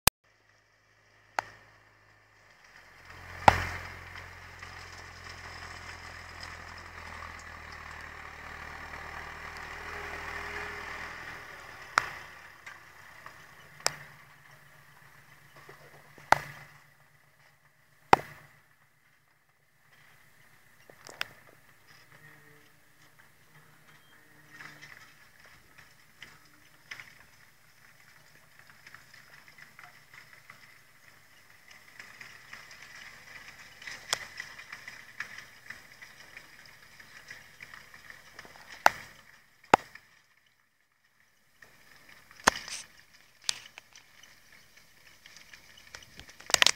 Landini Ghibli 95 Kuhn 7301
Ranghinatura secondo taglio erba medica